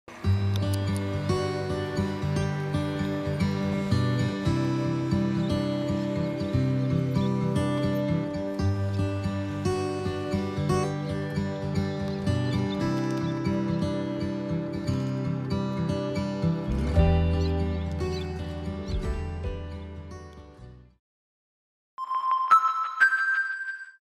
「アジュール舞子」に希少種コアジサシ 砂浜でせっせと子育て 神戸・垂水
海岸や河川敷に営巣する希少種コアジサシが、神戸市垂水区の公園「アジュール舞子」の砂浜に飛来し、子育てにいそしんでいる。